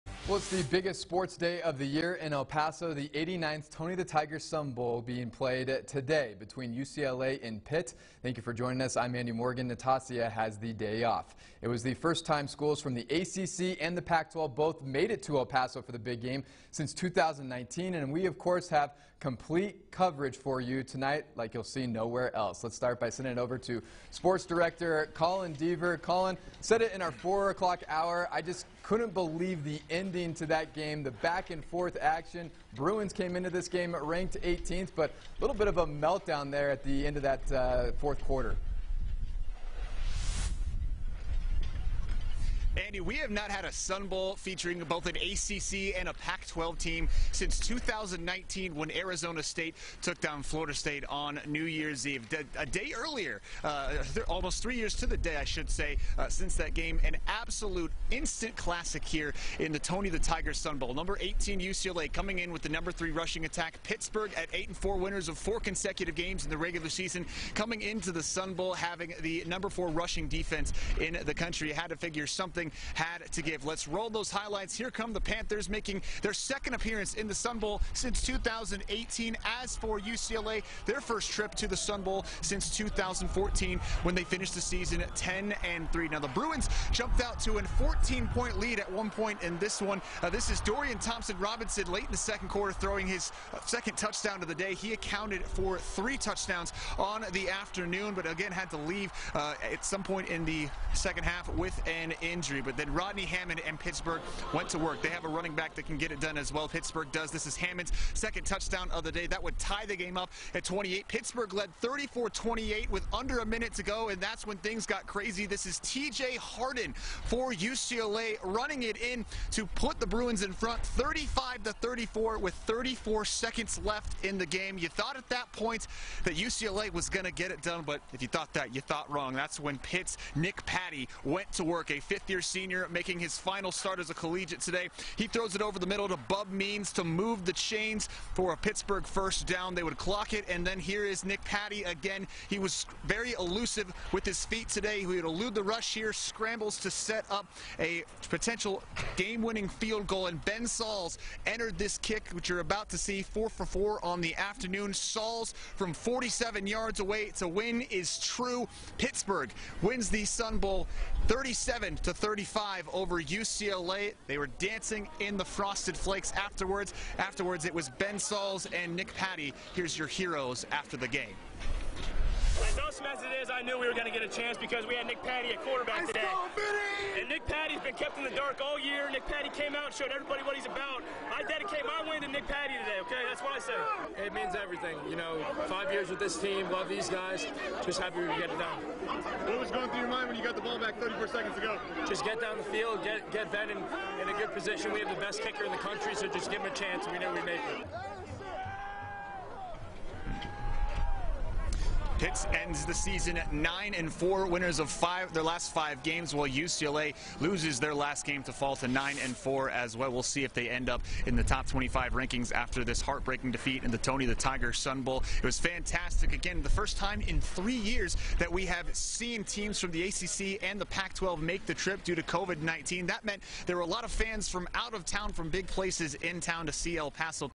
Pitt stuns UCLA 37-35 on game-winning field goal in Tony the Tiger Sun Bowl
Rodney Hammond Jr. rushed for 96 yards and two touchdowns, Pitt's defense forced four turnovers, while Ben Sauls kicked a 47-yard game-winning field goal, as the Panthers (9-4) overcame a 14-point deficit in the second half to beat UCLA (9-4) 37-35 on Friday in the 89th Tony the Tiger Sun Bowl at Sun Bowl Stadium.